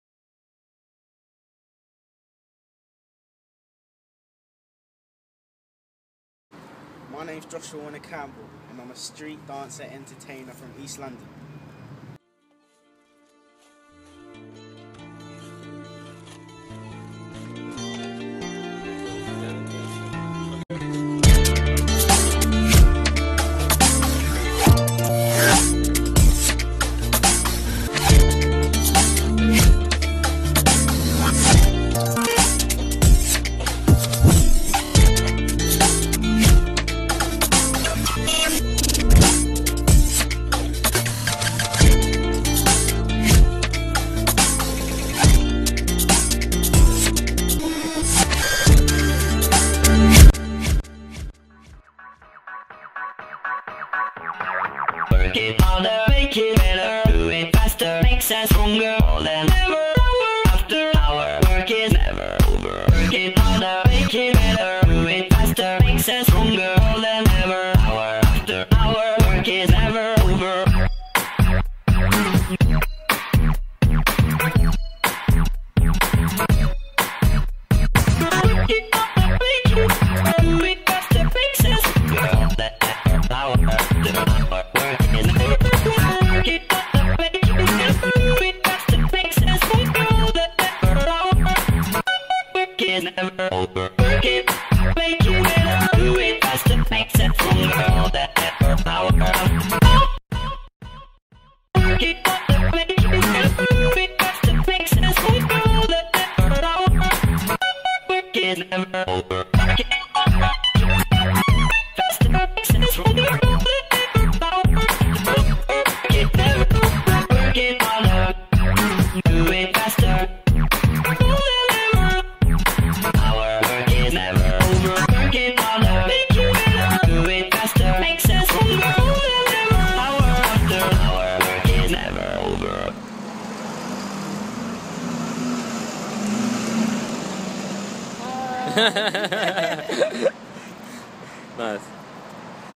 Introducing... Joshua Warner-Campbell
Josh has been dancing since the age of 7 and performed in street dance theatre shows and Gala's at the Young Actors Theatre Islington.

In 2009, Joshua was a contestant of Britain's Got Talent, reaching the semi-finals of the competition. He has collaborated with the Motion Dance Collective in their recent project 'Sunday...' and is surely looking to become the new hot dancer on the scene.

This video has been specially recorded for First News Diamond Kids Competition and has been done so entirely by iPhone 4s and edited on iMovie for Mac OS.